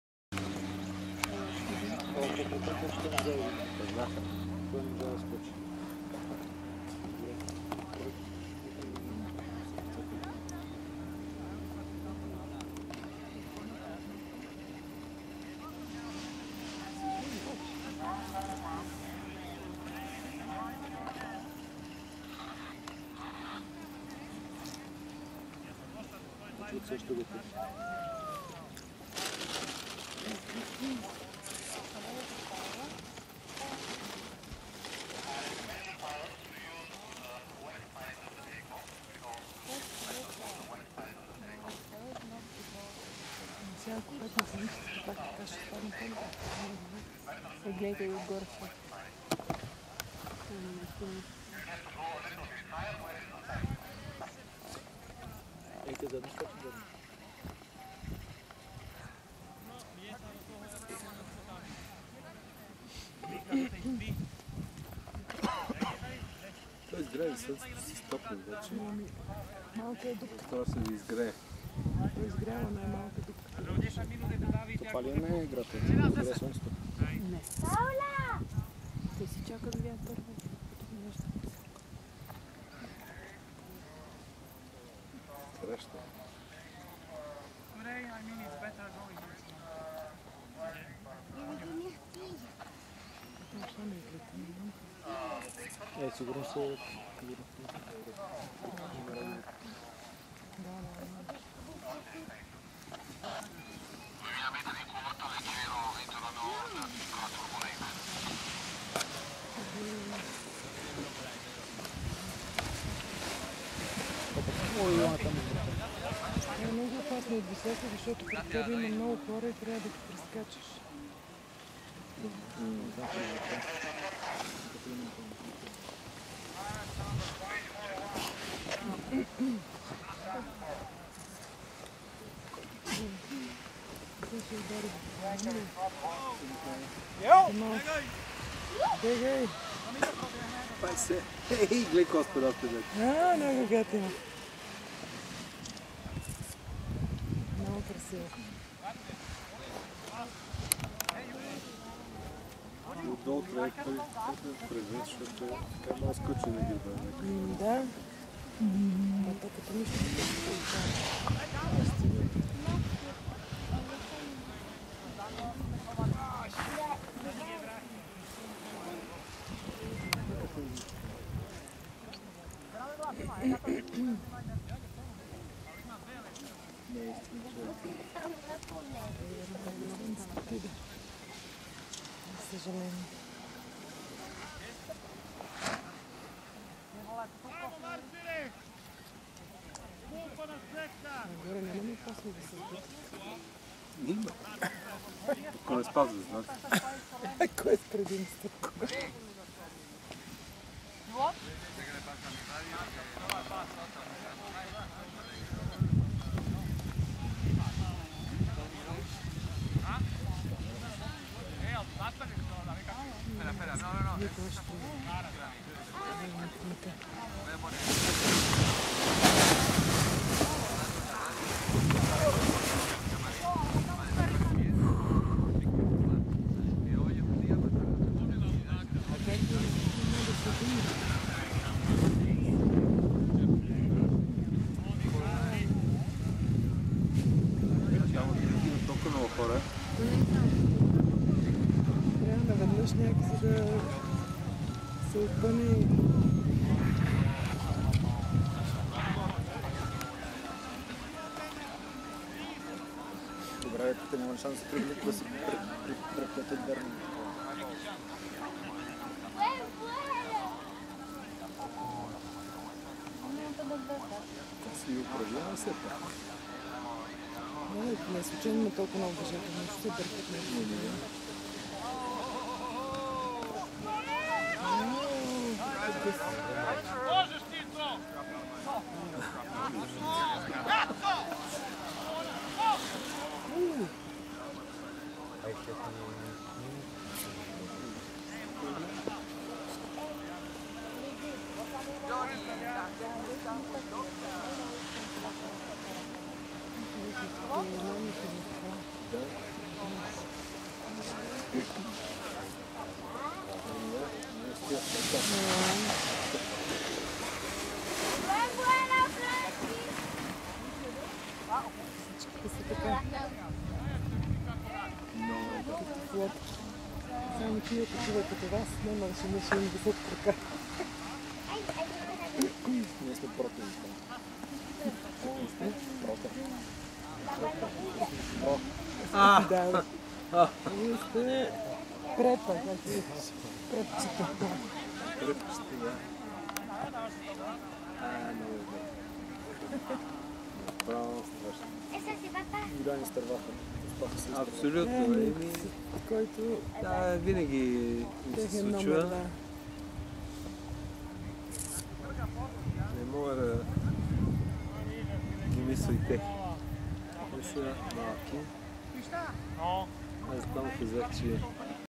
World Paragliding Championship Sopot Bulgaria - 21.07.2013
World Paragliding Championship Sopot Bulgaria - the start on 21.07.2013. Shortly after the start, the race was cancelled due to bad weather. It was amazing to see the sky filled with so many paragliders.